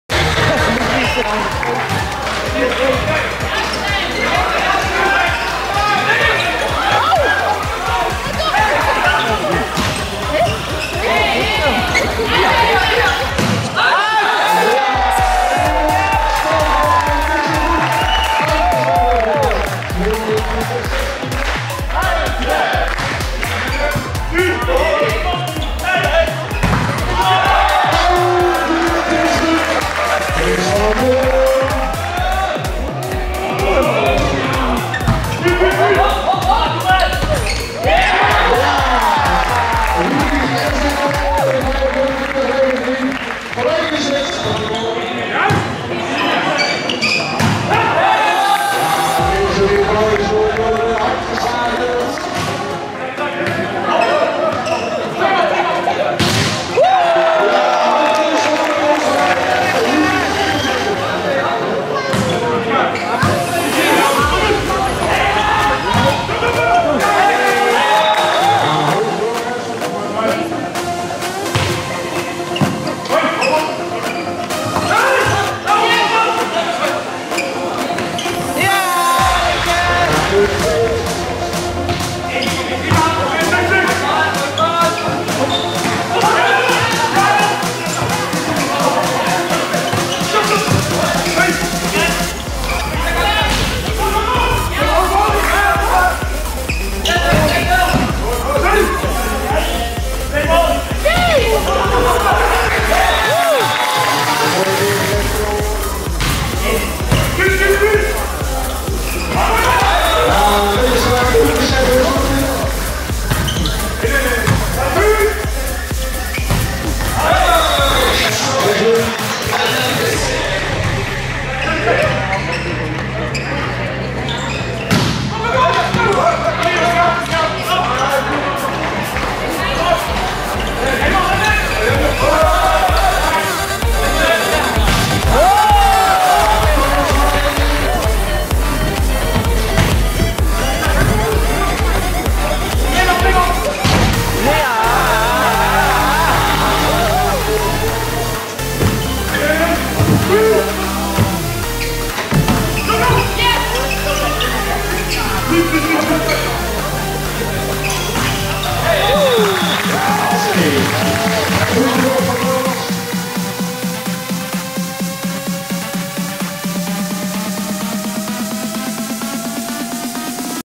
CAS-CRM /ZVH - VoCASA 04112017
Videobeelden uit de Topdivisiewedstrijd CAS-CRM/ZVH H1 - VoCASA H1 gespeeld op 4 November 2017 in Swanla